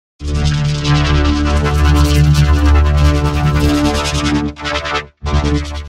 An Audio Effect I'll Call "Batteryup B"
(VideoFX)
TV Simulator: Line Sync 7500
NewBlueME Wiggle: Flag but edges is stretch and strength is 67
Gradient Map: Batteryup B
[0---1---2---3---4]
0: White
1: Blue
2: Green
3: Black
4: Cyan
(AudioFX)
Chorus: Crazy Diamond
ExpressFX Chorus: Weird Code
IL Vocodex:
Preset: Power
Bandwidth: Max
Keys: 13rd White Key